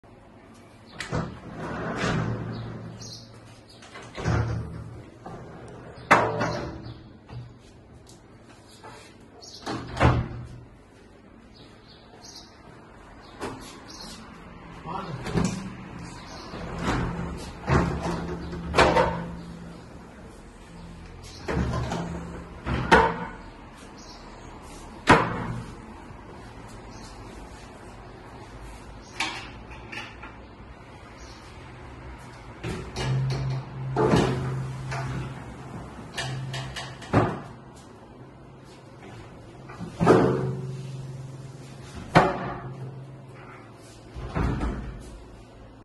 Inchidere terasa cu sticla securizata glisanta
Sticla securizata glisanta -10 mm
Banda magnetica